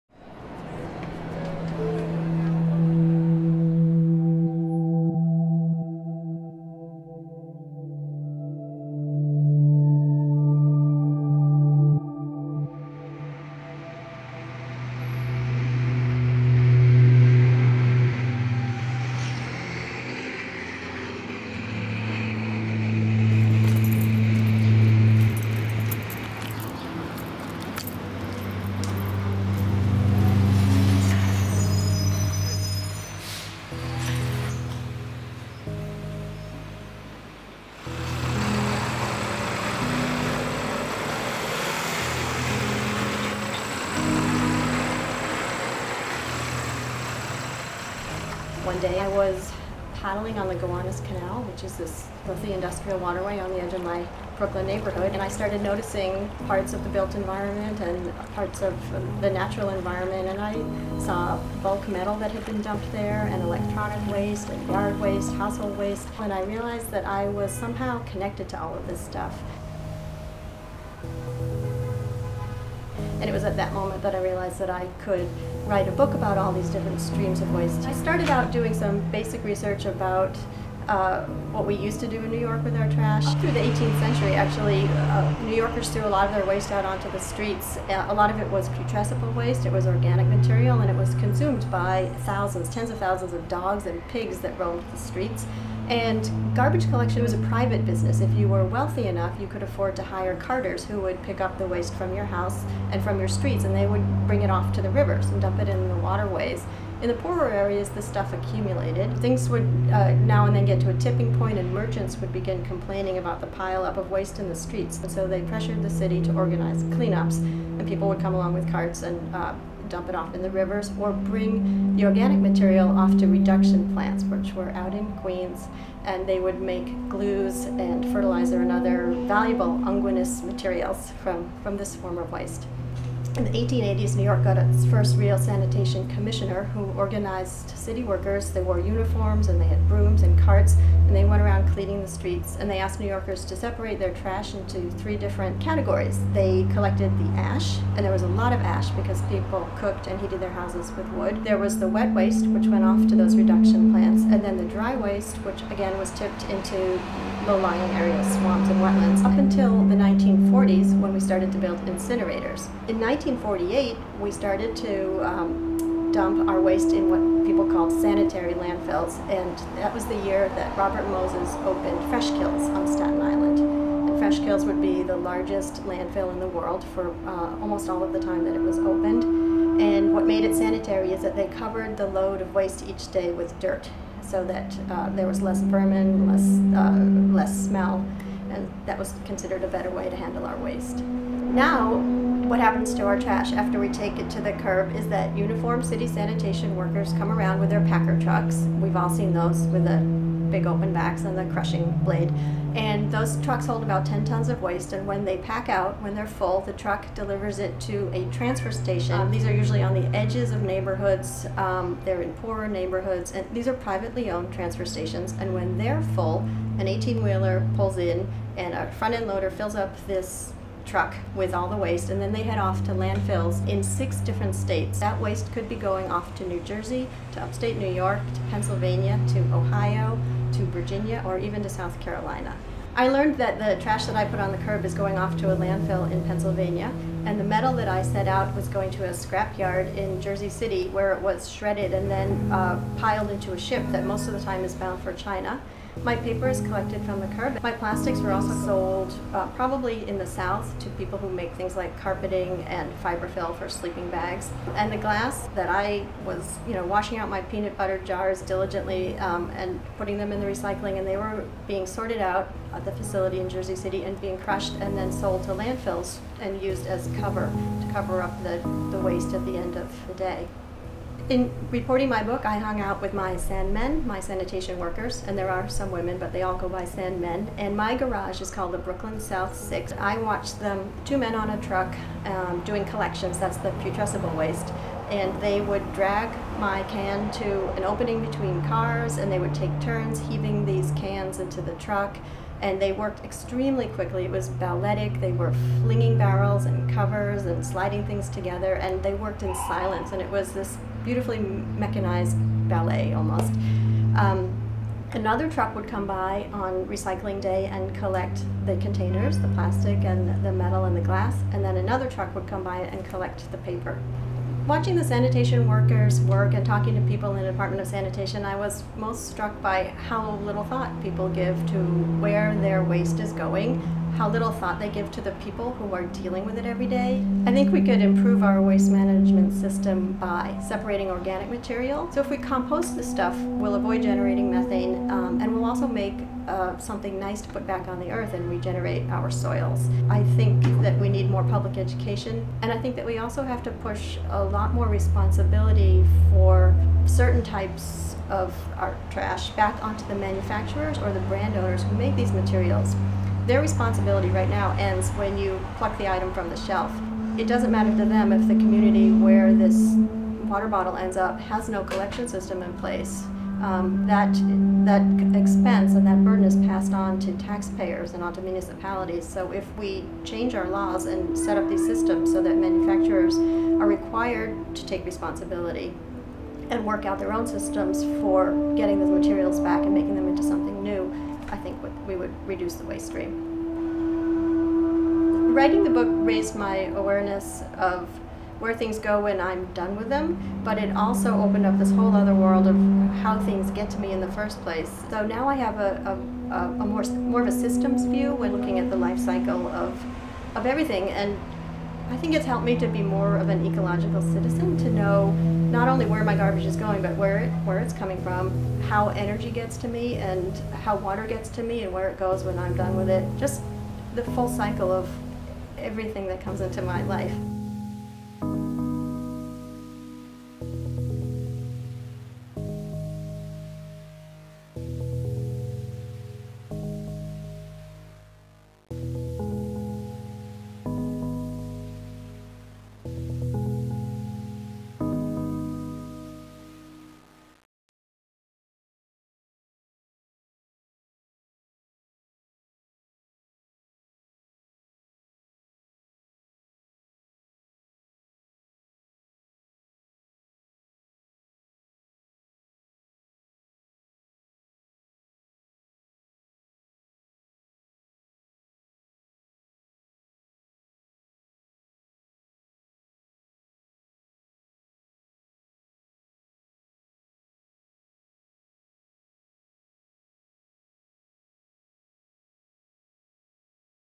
City of Systems: Waste Removal
In our final video on complex urban systems, writer Elizabeth Royte offers a snapshot of how New Yorkers have treated their trash from the 17th Century onwards, describing how her research has led her to become a more ecological citizen, with "a systems view" of our interconnected processes of manufacturing, transportation, disposal and re-use.